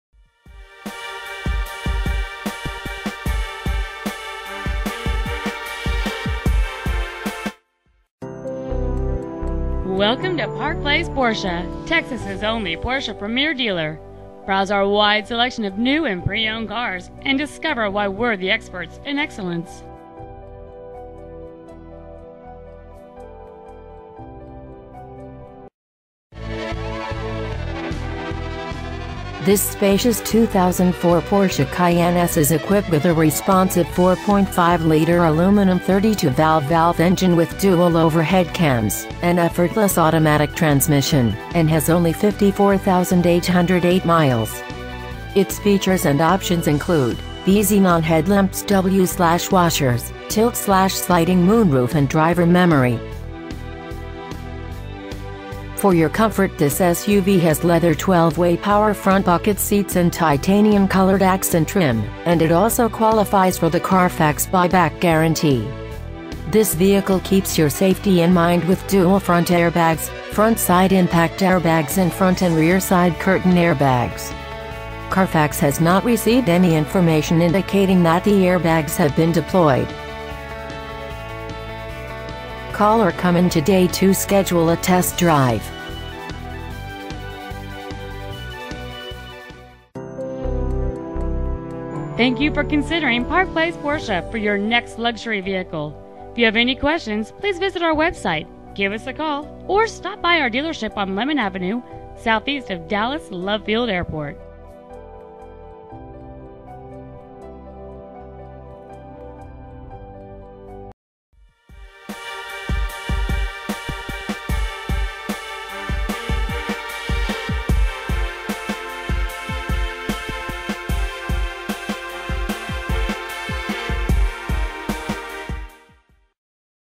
Preowned 2004 Porsche Cayenne Ft Worth Texas 75209
Park Place Porsche
 6113 Lemon Ave
 Dallas, Texas 75209

 Year: 2004
 Make: Porsche
 Model: Cayenne
 Engine: 4.5L (275) DOHC SMPI aluminum water-cooled 32-valve V8 engine
 Trans.: Automatic
 Exterior: Other
 Miles: 54,808
 Interior: Other